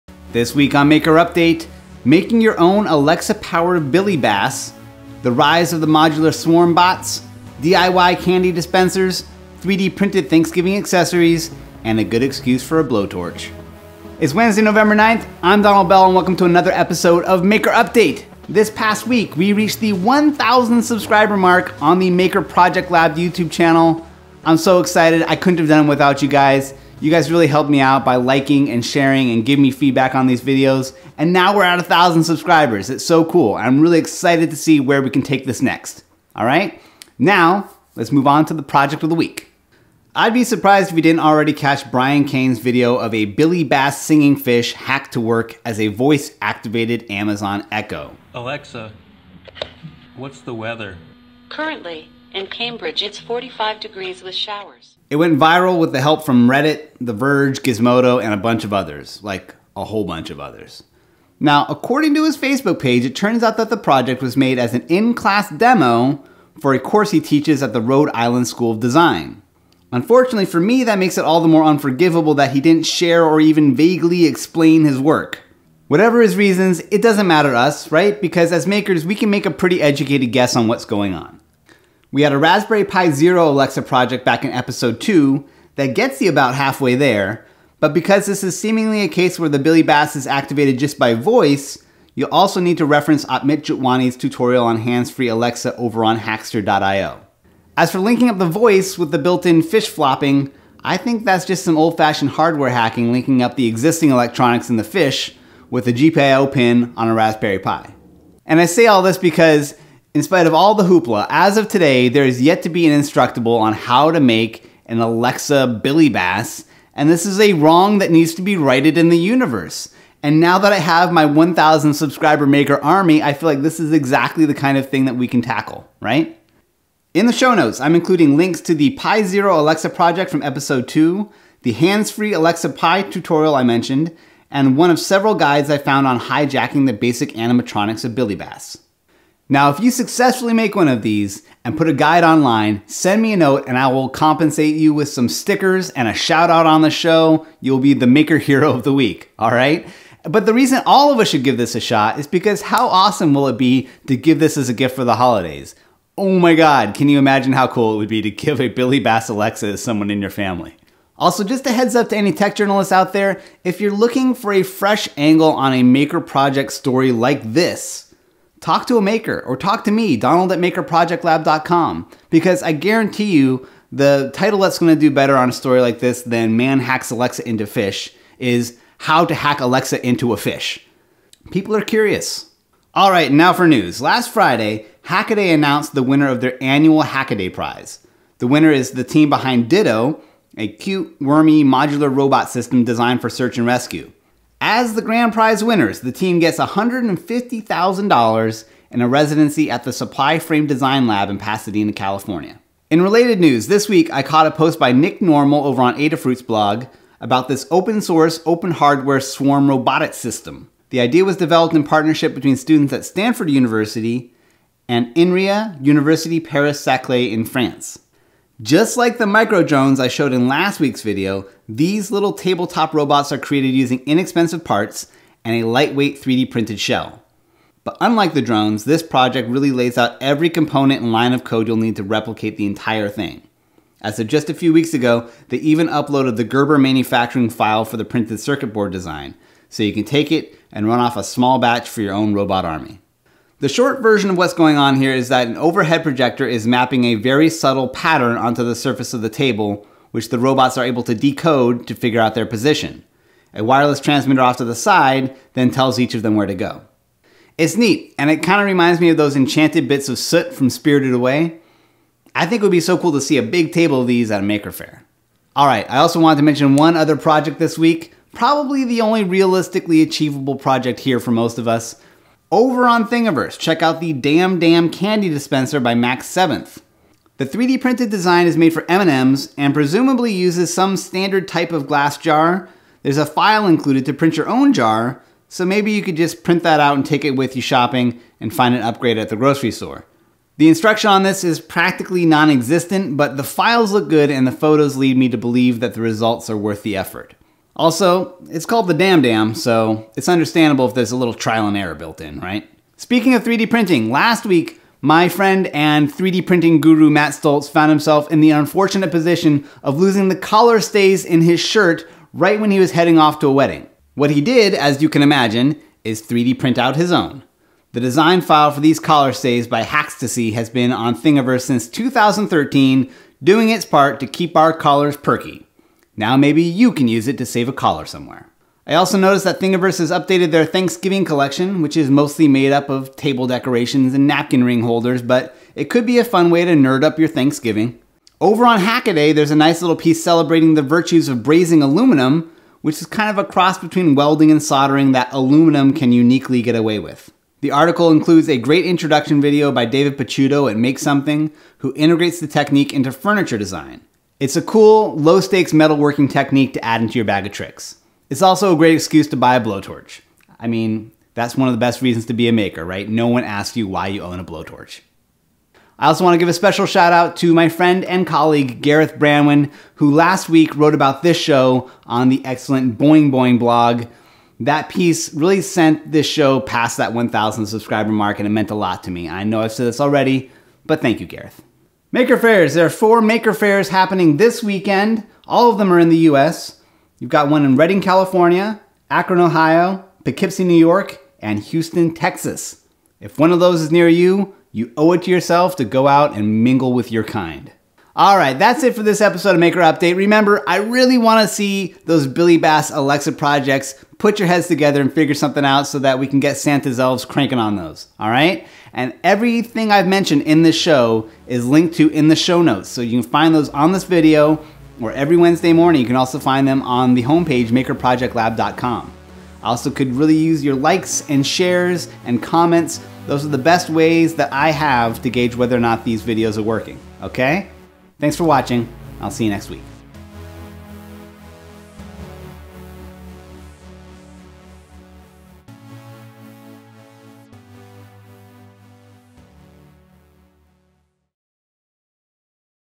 Let’s Hack Alexa into a Fish [Maker Update #10]
This week on Maker Update: making your own Alexa-Powered Billy Bass, the rise of the modular swarm bots, DIY candy dispensers, 3D printed Thanksgiving accessories, and a good excuse for a blow torch.

-=-=Show Notes=-=-

News
Dtto wins Hackaday Prize

‘Zooids’ are Open-Source, Open-Hardware ‘Bots for ‘Swarm User Interfaces’

Projects
Dam Dam Candy Dispenser

Tools/Tips
Collar Stays

Shoutout
Thanks Gareth and Boing Boing

Shasta County Mini Maker Faire  Redding, California USA
Akron Mini Maker Faire  Akron, Ohio USA
Poughkeepsie Mini Maker Faire  Poughkeepsie, New York USA
Houston Mini Maker Faire  Houston, Texas USA